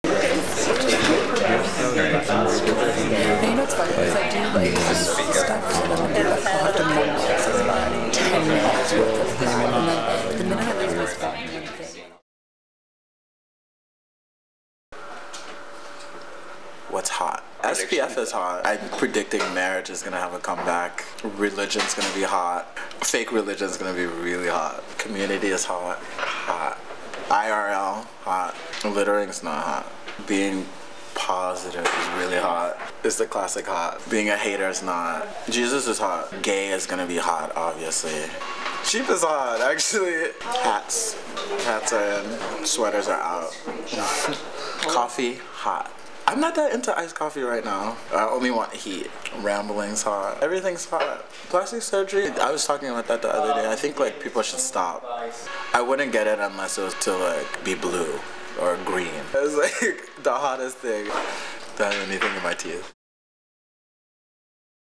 Hot & Not with Telfar | VFILES TMI
Hot tips for the summer from designer Telfar Clemens.